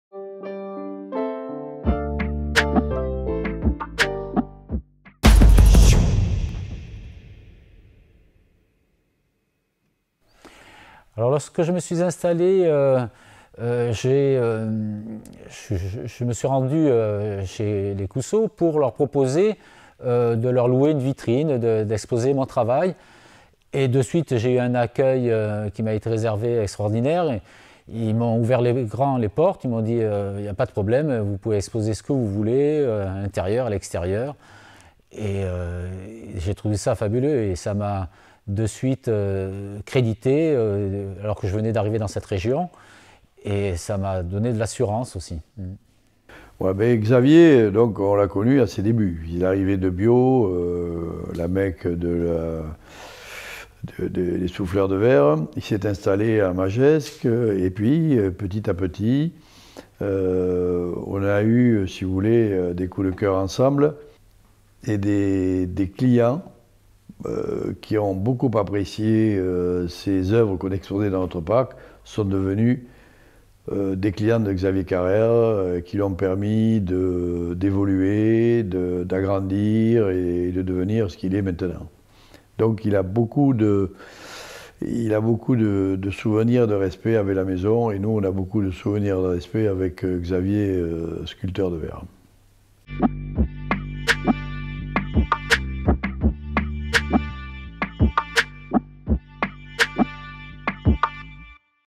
L’artiste sculpteur Xavier Carrère ami de la Maison Coussau depuis 25 ans
En 25 ans, l’artiste souffleur de verre et sculpteur, Xavier Carrère, est devenu un véritable ami de la famille Coussau et du Relais de la Poste.

Une fois encore cette année, les œuvres de Xavier Carrère sont à retrouver sur les murs de la salle du restaurant doublement étoilé au Guide Michelin du Relais de la Poste mais aussi dans le parc de la Maison Coussau ou l’artiste expose ses œuvres monumentales.